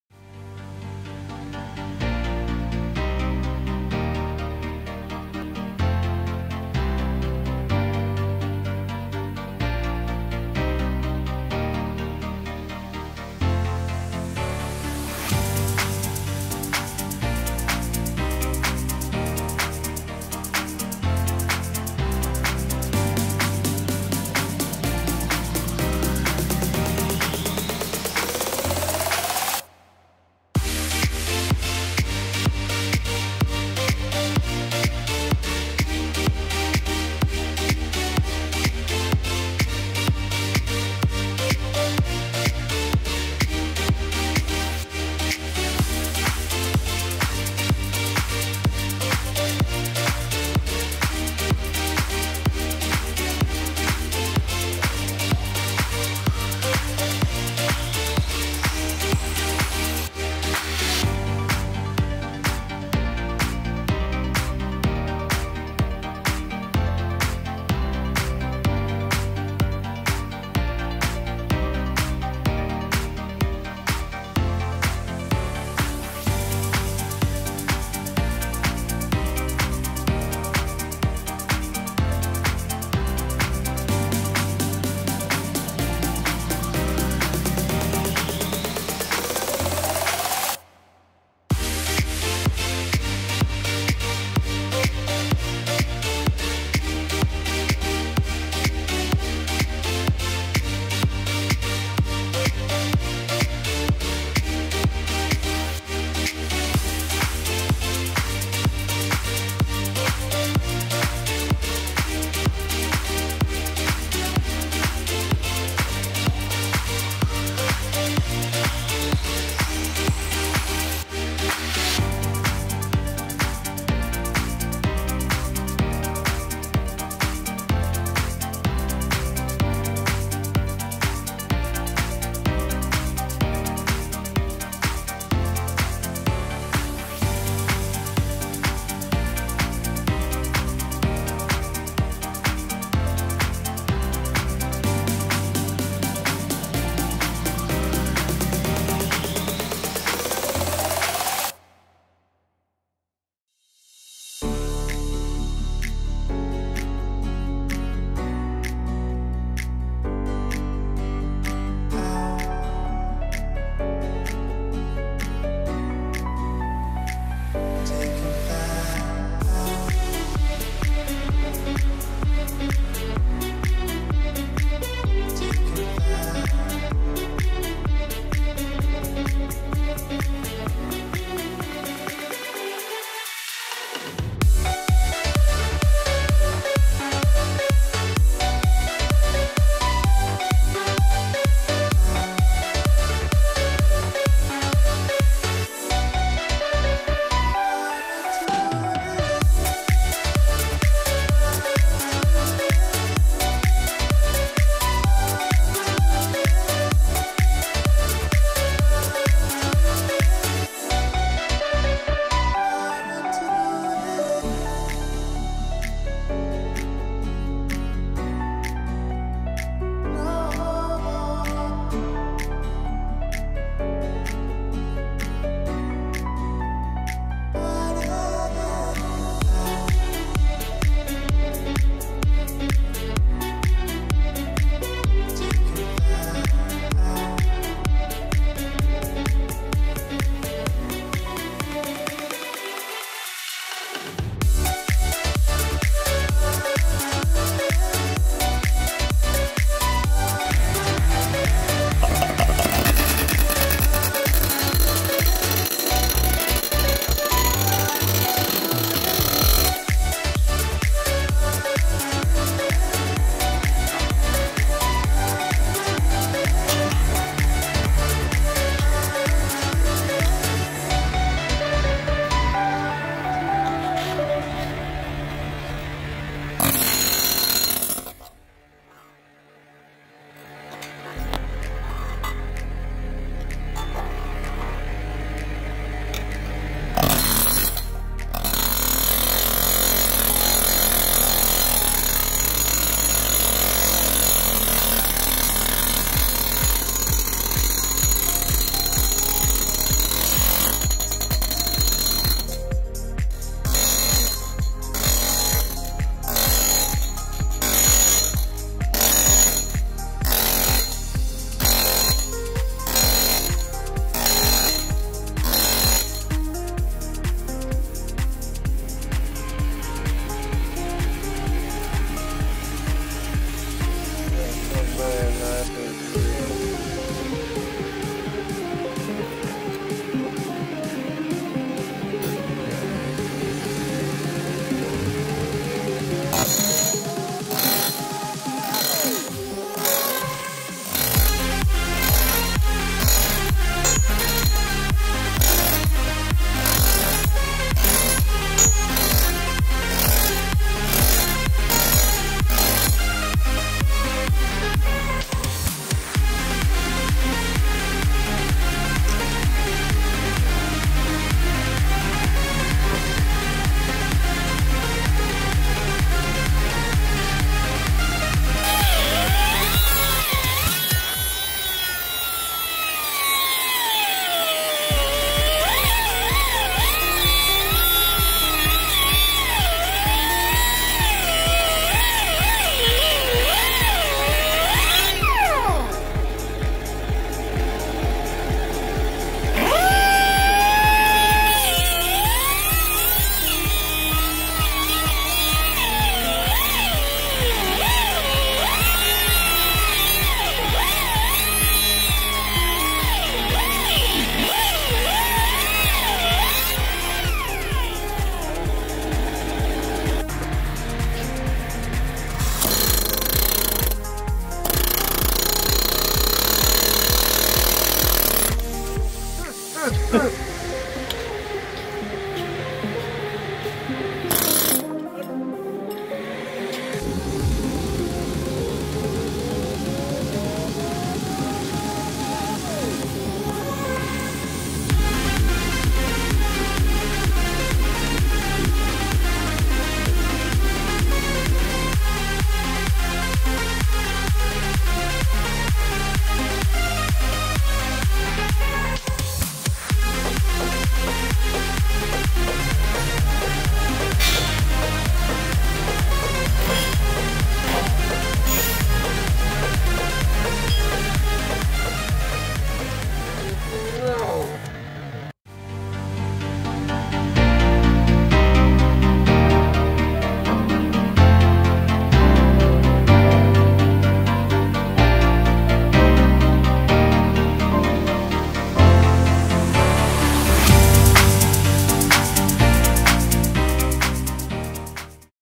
Toyota Sienna Rusted CV Joint Axle easy removal / DadakitsTV
Easy way to remove rusted cv joint axle of Toyota Sienna,, not to waste time and effort.. "time is gold, time is money"